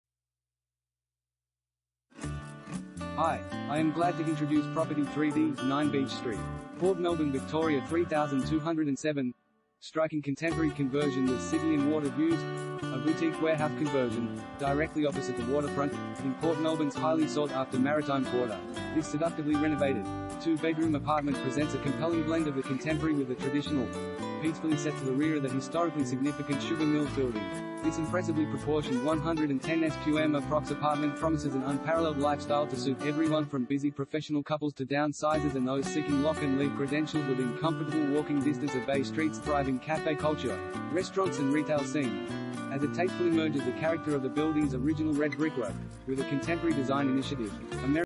3D/9 Beach Street, Port Melbourne VIC 3207 | AuPropertyReport.Com
3D/9 Beach Street, Port Melbourne VIC 3207
Property Type: apartment
Bedrooms: 2
Bathrooms: 2
Car Space: 1
Striking Contemporary Conversion with City and Water Views